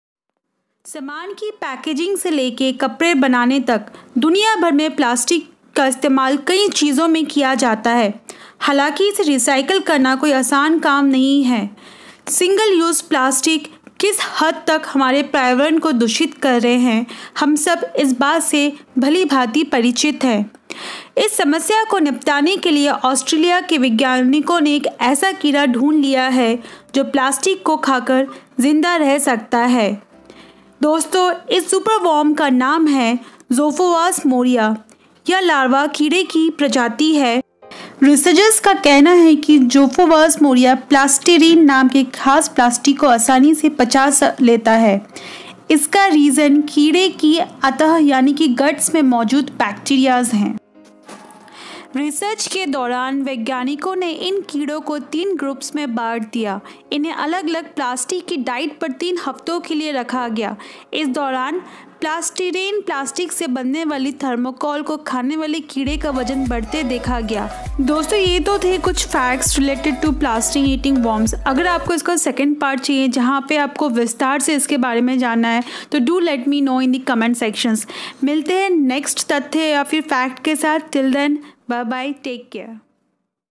क्या ये कीड़े प्लास्टिक से मुक्ति दिला सकते हैं| Plastic eating worm| Educational facts
Plastic takes hundreds of years to be destroyed, but it will never completely disappear. The result of this will be microplastics - such small particles of plastic that you cannot see with the naked eye and they cause great harm to the environment. How would it be if after use we could even throttle it… Scientists have found its solution in nature itself.